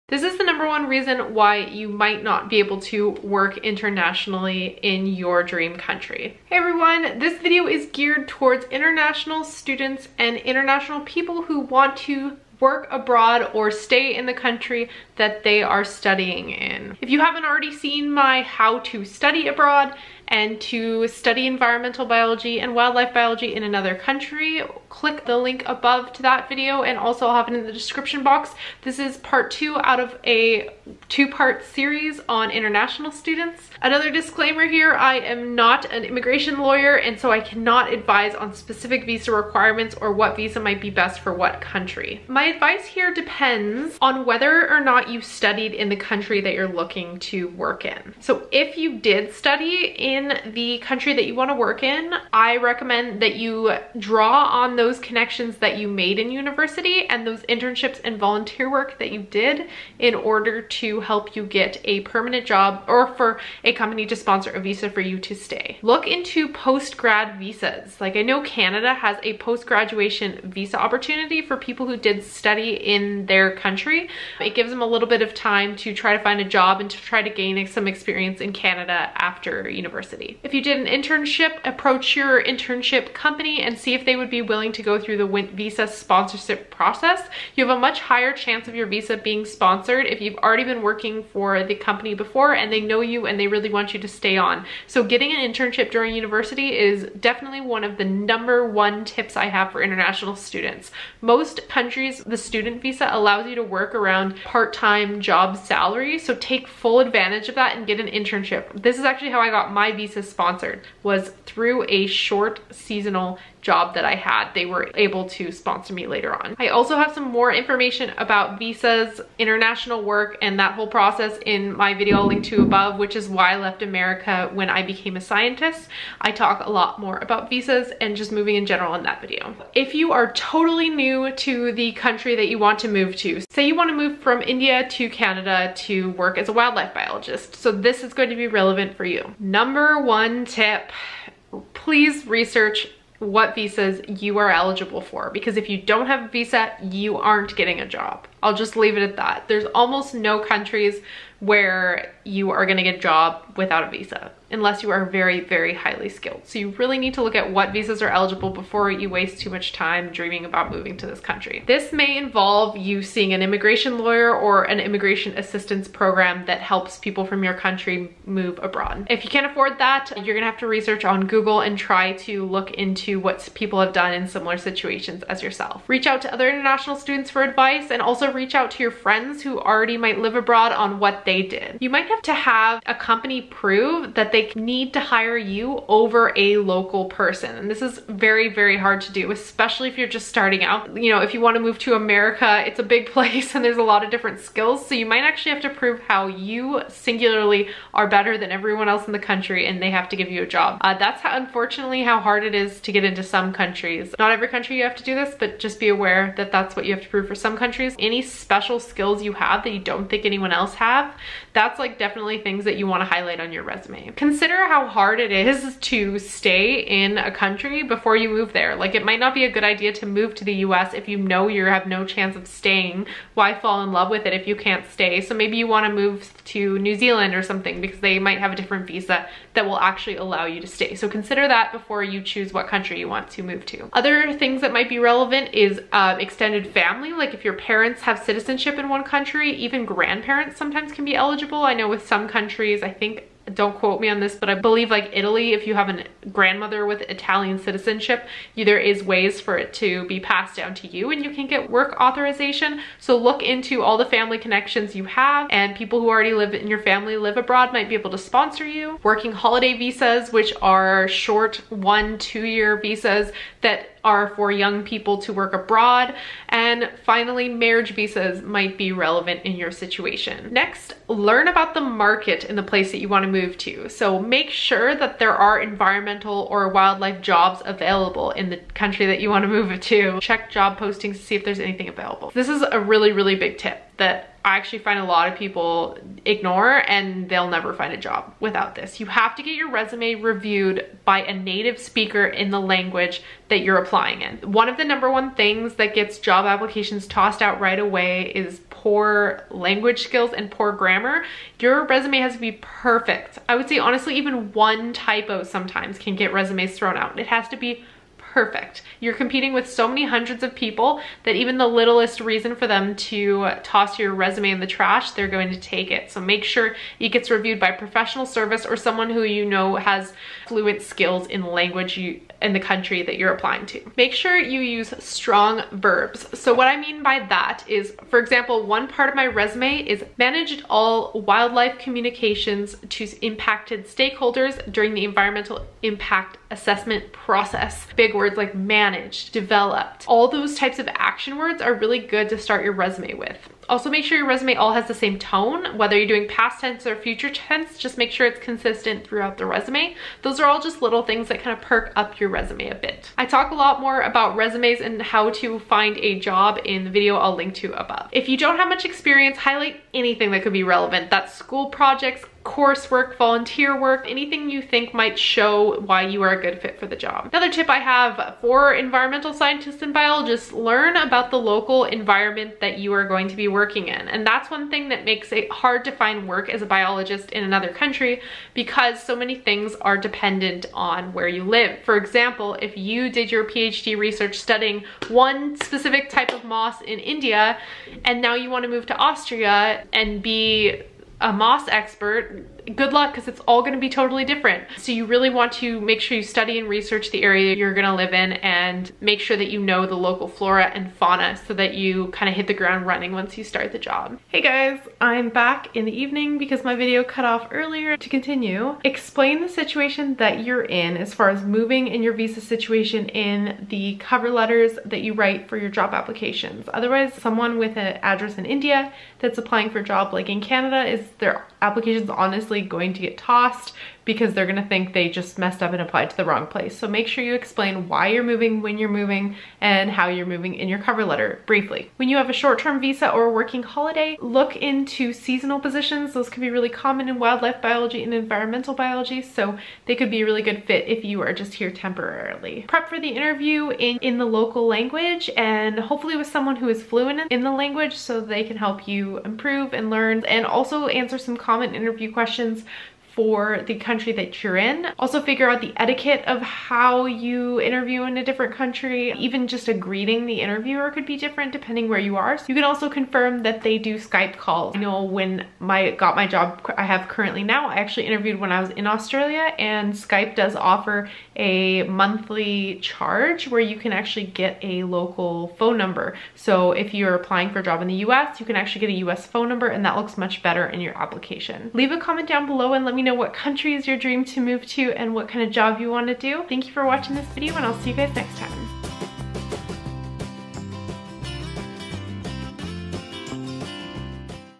WORK abroad in environmental science!
This is your complete guide on how to work abroad in the field of environmental science and wildlife biology, coming at it from the lens of a recent international graduate - but it is relevant for anyone wanting to work abroad! I go through interviewing abroad, how to structure your resume, cover letter, visas, and some ESSENTIAL things you need to know otherwise you will just be endlessly applying with no end in sight...